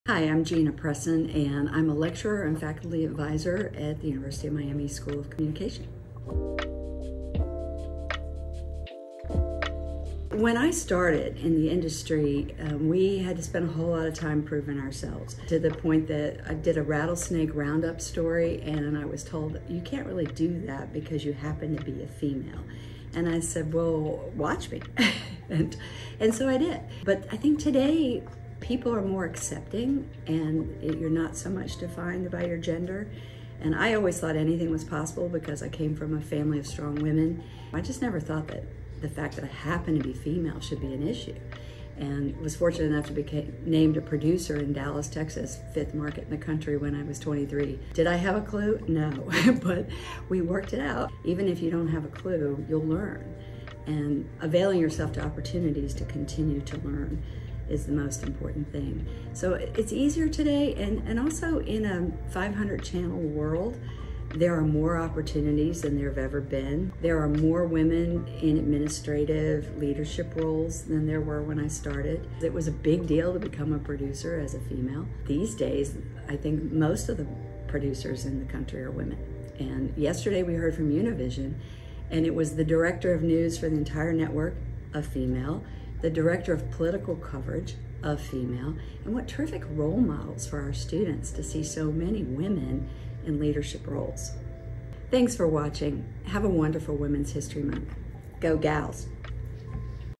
Women's History Month 2023: Gina Presson
Longtime reporter and producer turned Lecturer and UMTV Faculty Advisor, Gina Presson, joins our WomensHistoryMonth series! 🎥

Professor Presson reflects on the challenges and opportunities in her early career and how the field of BroadcastJournalism has transformed as an industry.